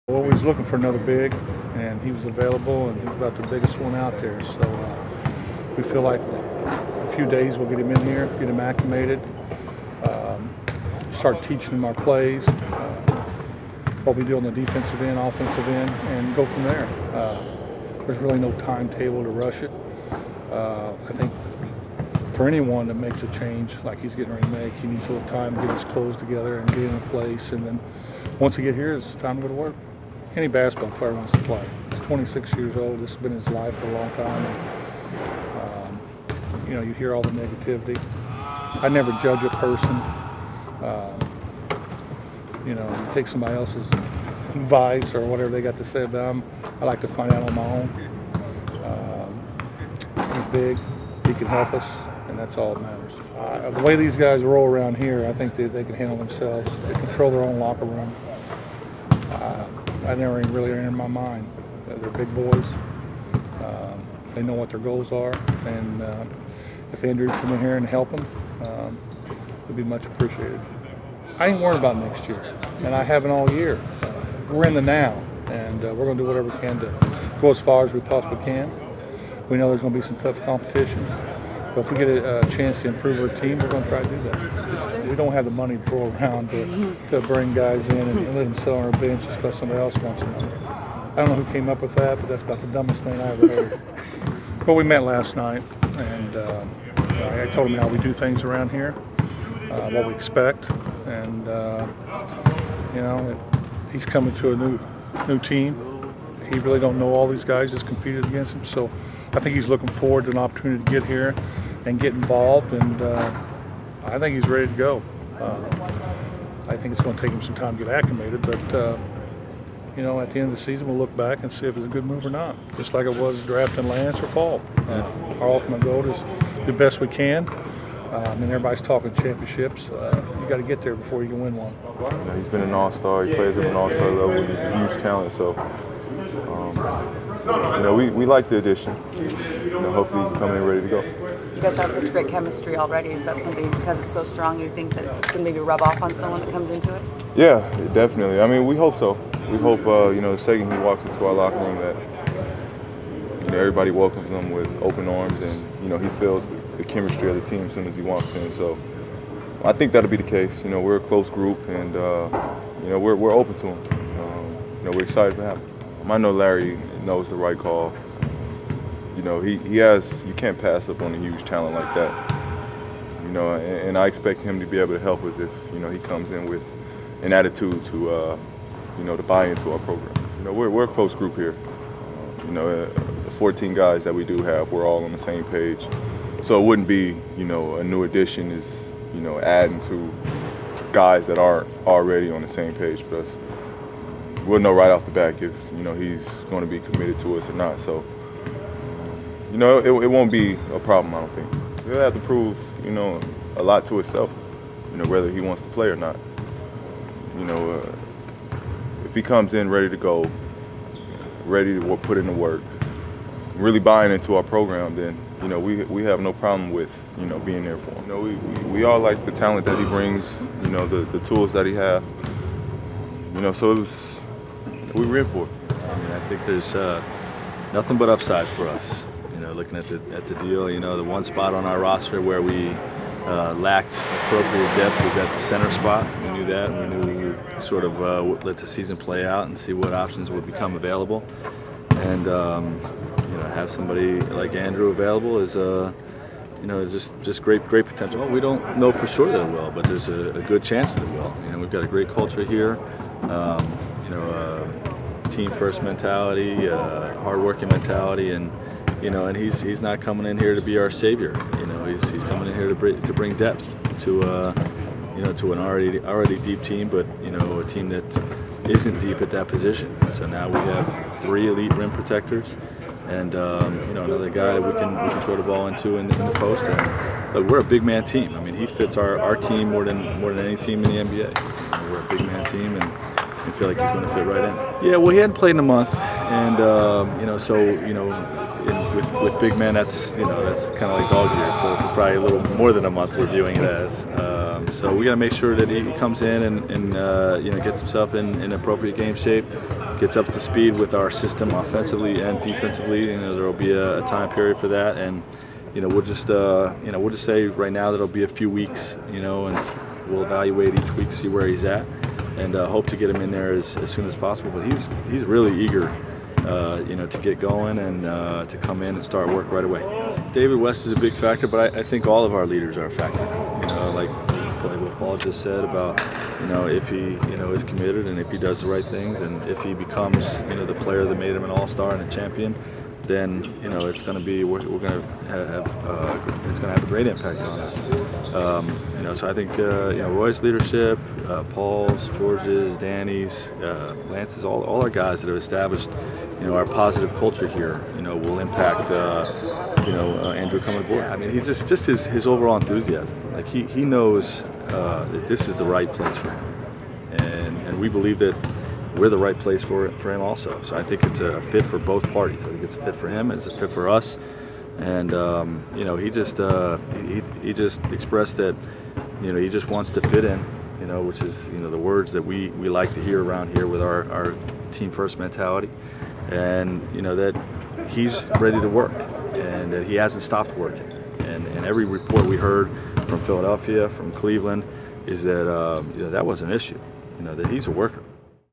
Bird, Vogel, and George on signing of Andrew Bynum
Larry Bird, Paul George, and Frank Vogel on the signing of mercuial but talented center Andrew Bynum.